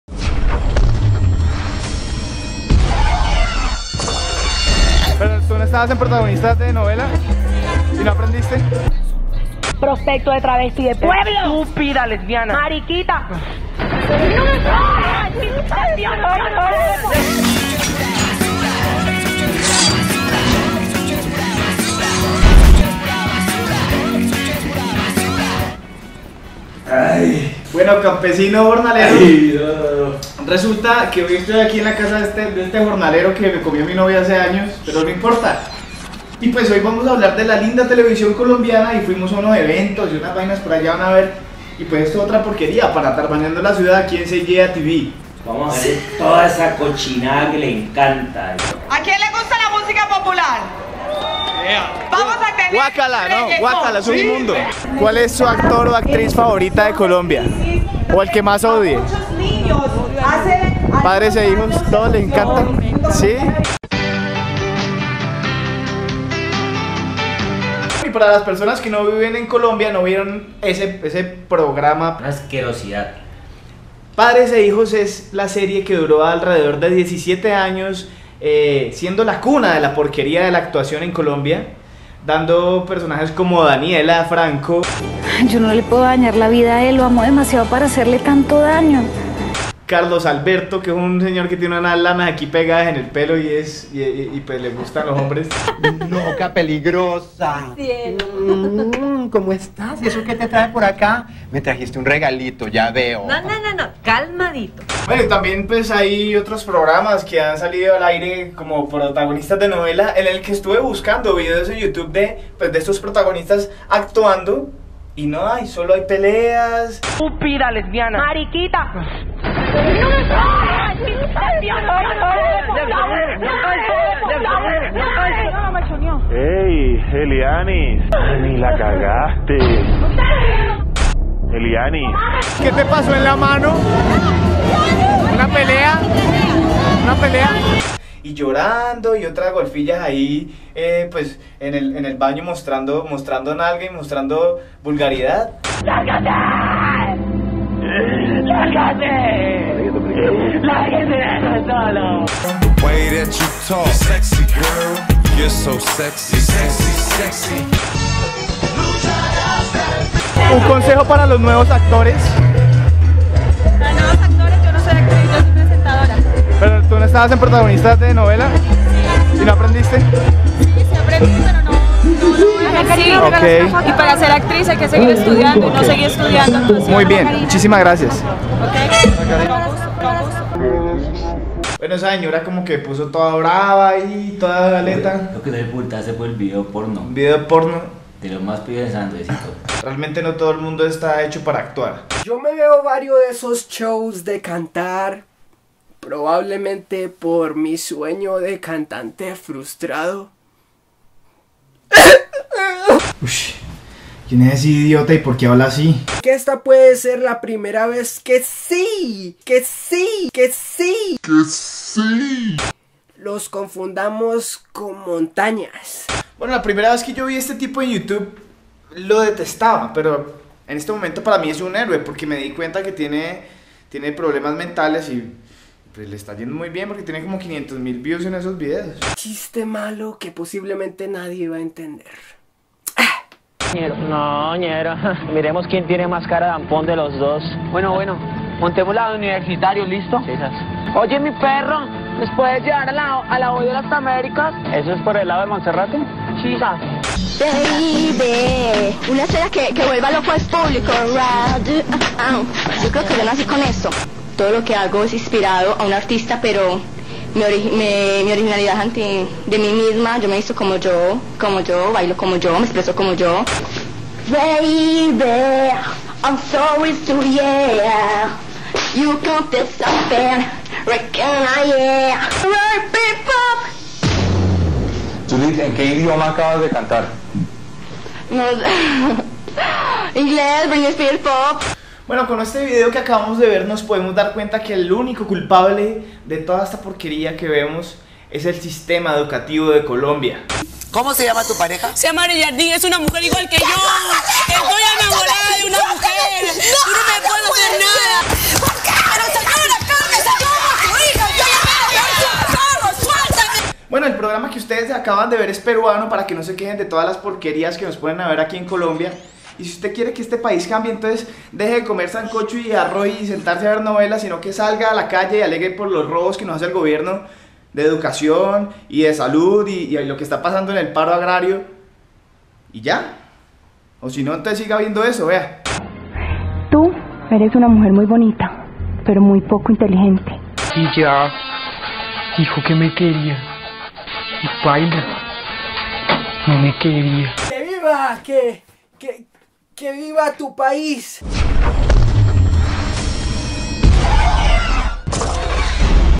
La Televisión Basura / Atarbaniando La Ciudad
@DavidZunigaA se sumerge en un evento de Actores para hablar de la televisión (basura).